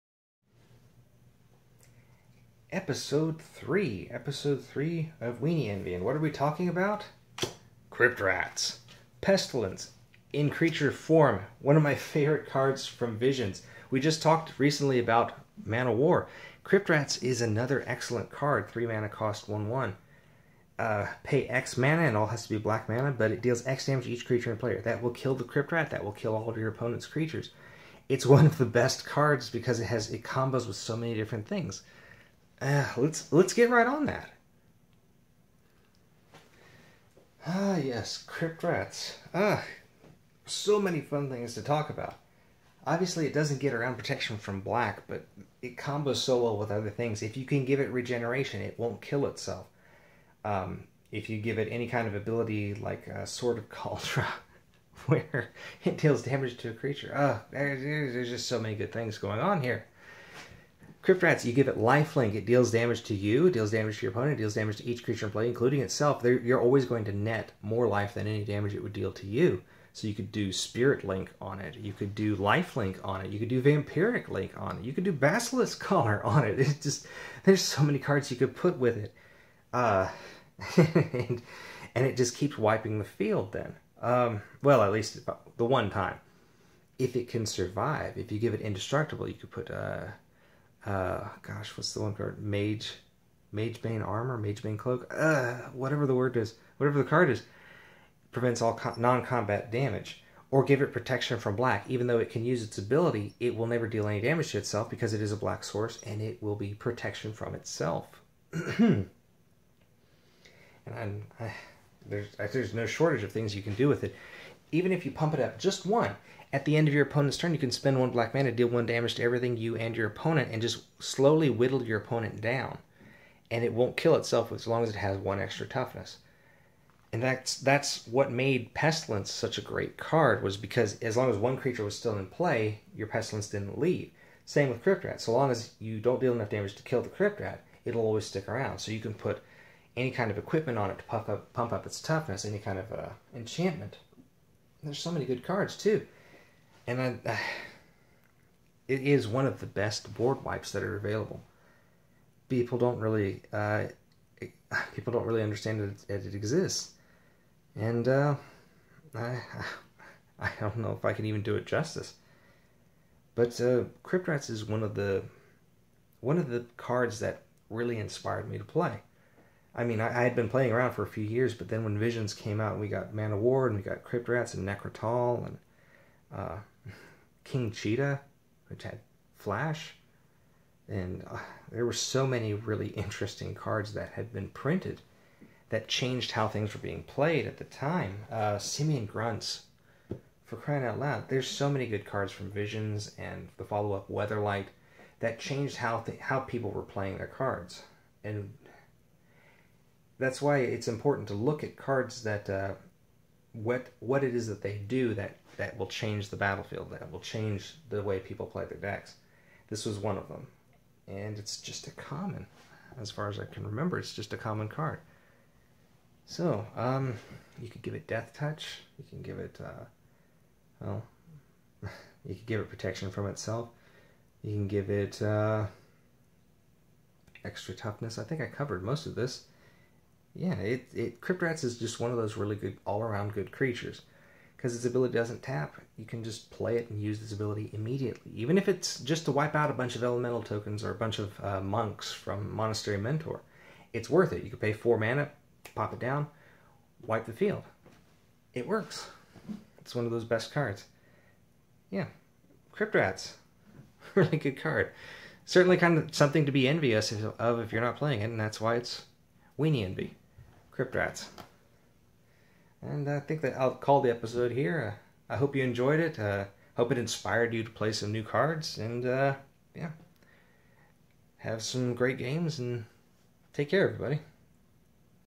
Weenie Envy: Crypt Rats! Pestilence On Four Legs!
In days of old, when you wanted to wipe the field of token creatures, there was one 1/1 tiny titan of removal. In Jamuura, Crypt Rat exterminates EVERYTHING ELSE.